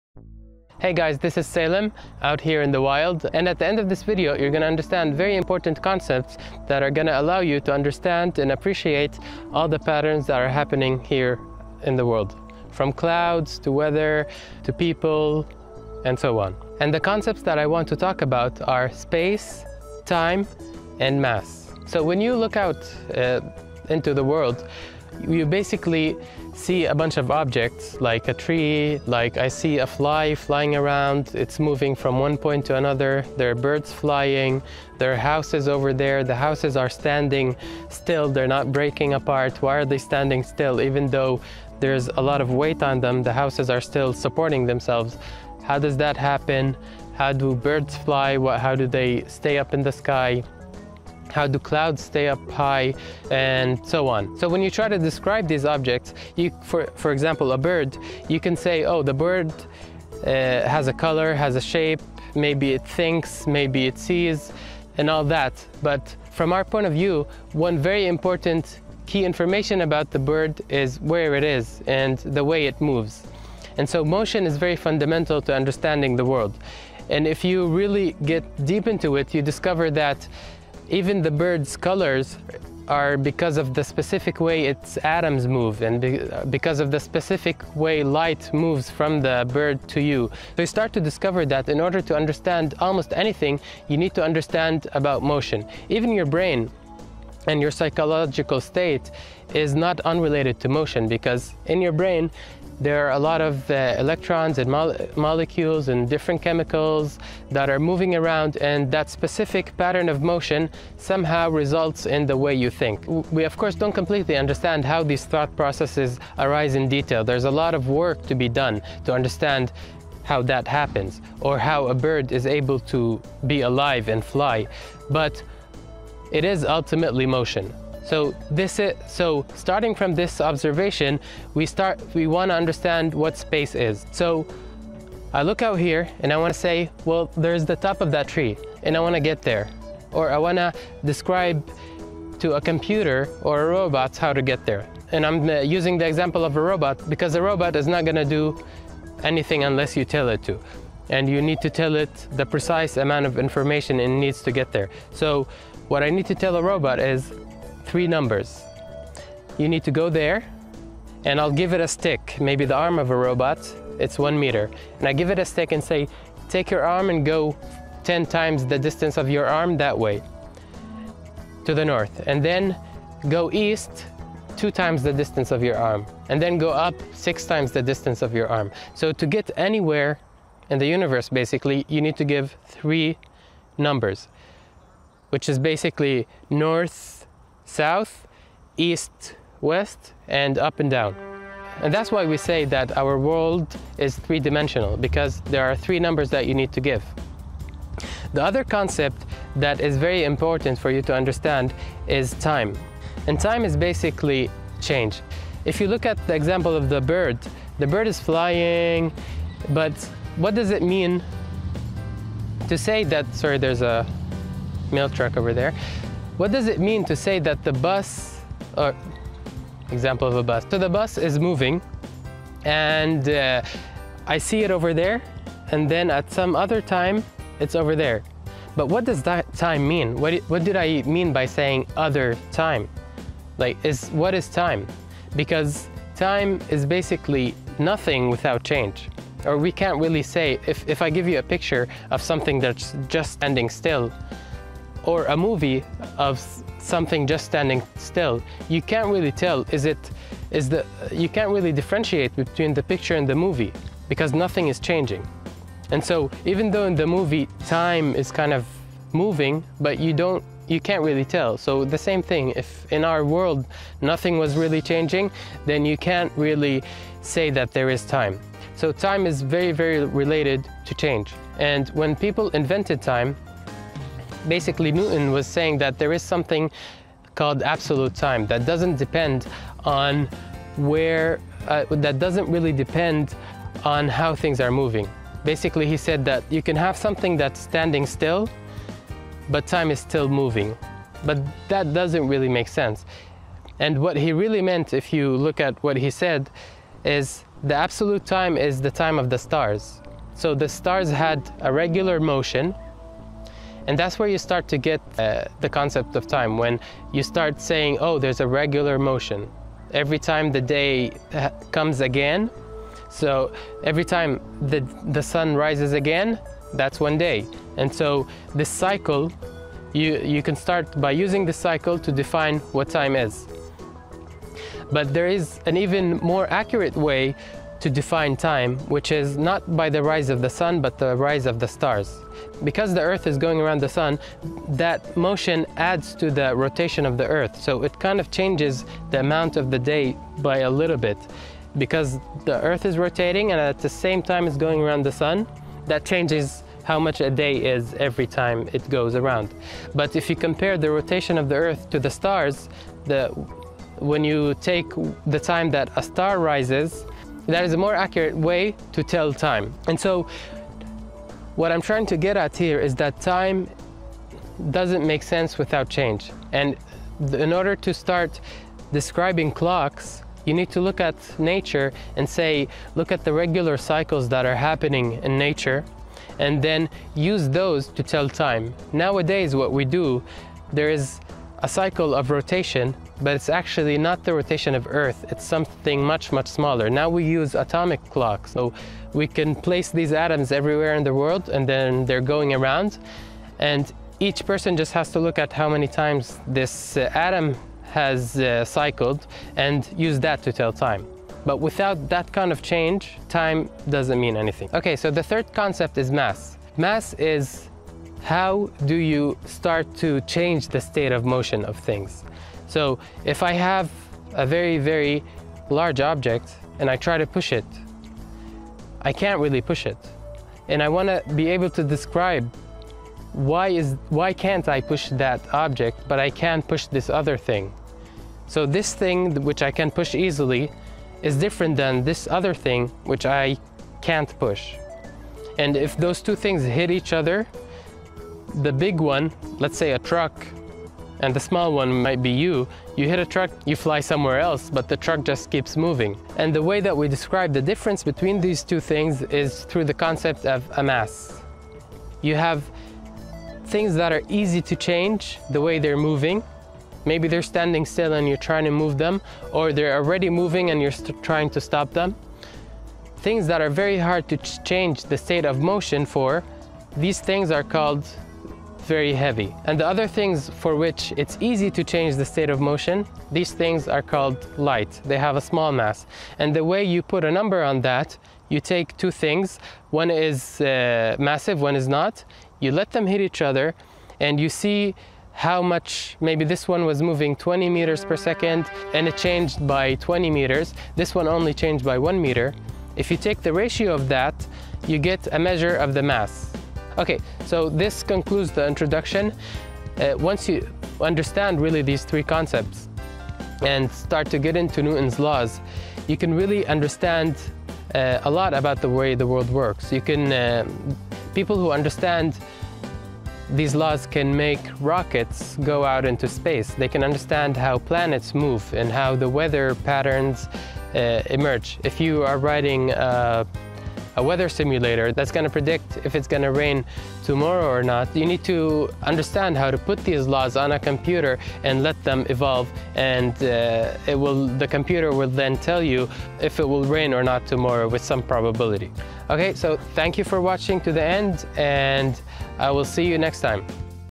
Space, Time and Mass in Classical Physics.
In this video, YOU will understand the three most important concepts in physics, especially classical physics and Newtonian Mechanics. Understanding these concepts is crucial for an appreciation of our world.

It is also critical for so many practical applications. If you want to be someone that know how rockets are made, or how planets orbit the sun. Or if you wanna work in making weather simulations, or design cars or quantum computers, you will need to understand about space, time and mass.